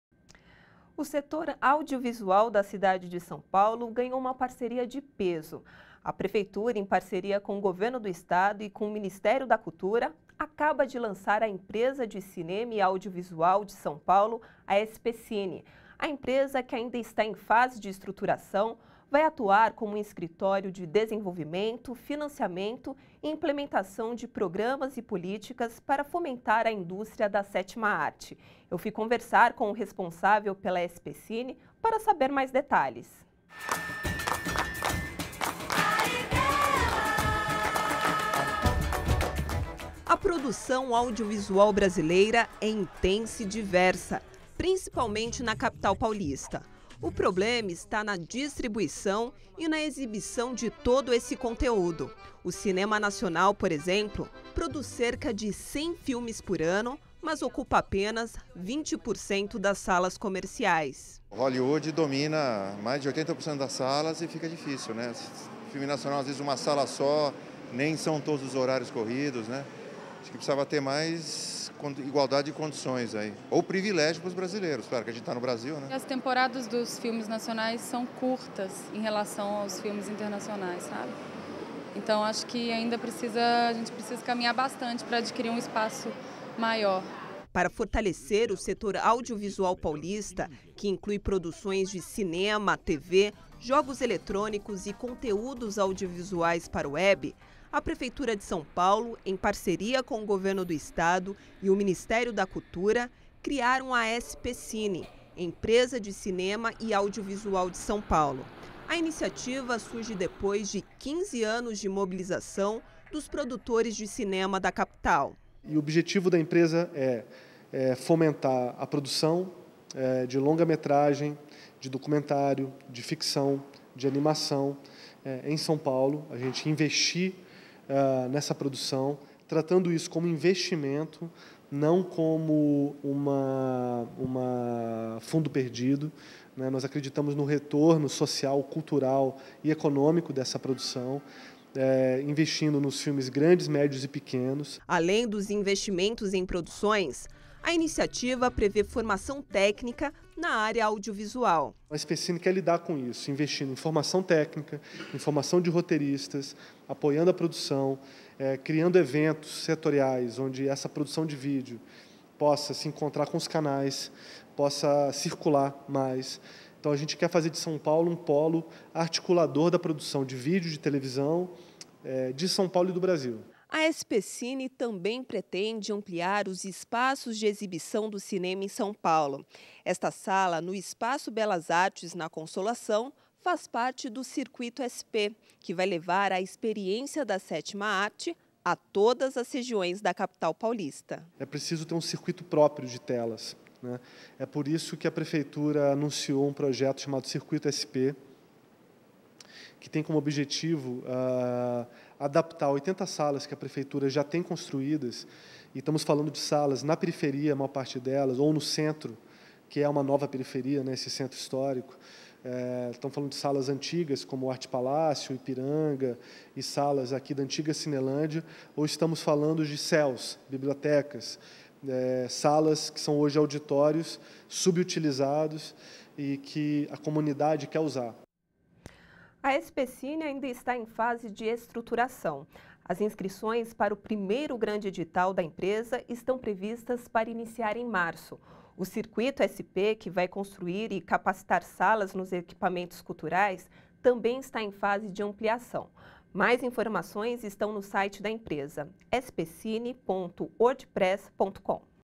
Setor audivisual ganha parceria de peso em SP: conheça a Spcine
A empresa vai atuar como um escritório de desenvolvimento, financiamento e implementação de programas para fomentar a indústria da sétima arte em SP.